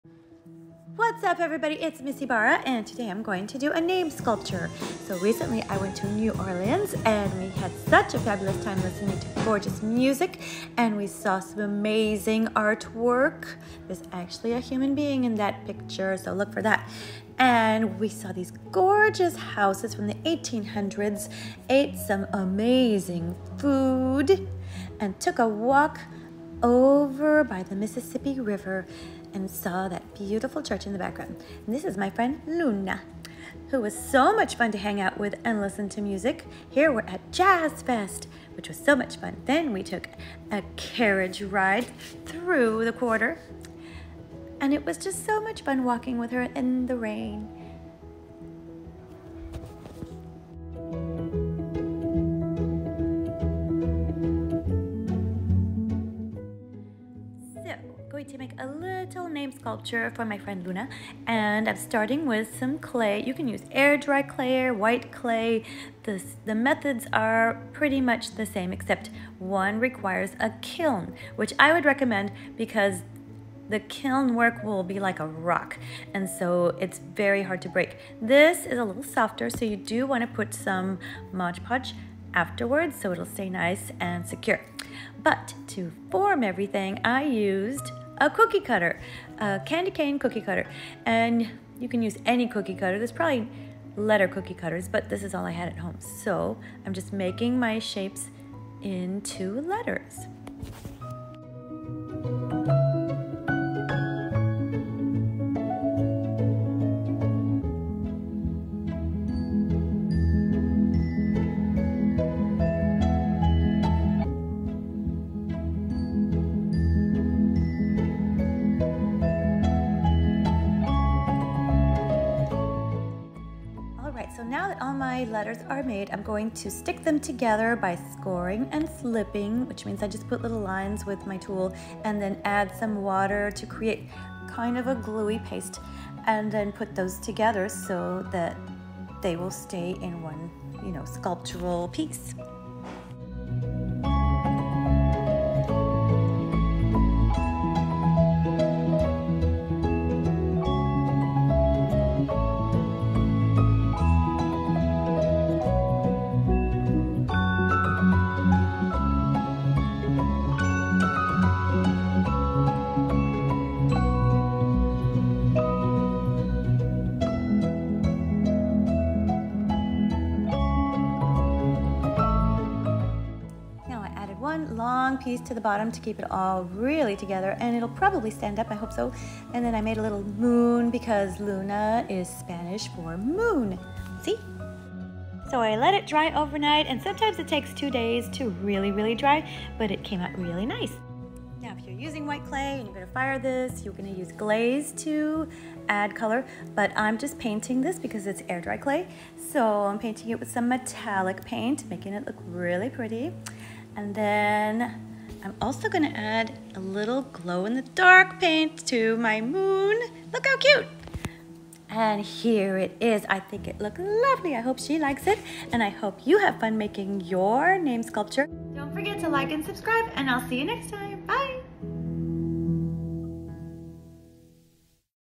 I went to New Orleans then made a name sculpture for my travel buddy!
🌙Here is what I used for this project!

Air dry clay:

Clay tools:

Here’s white clay if you are using a kiln!

And if you need a kiln….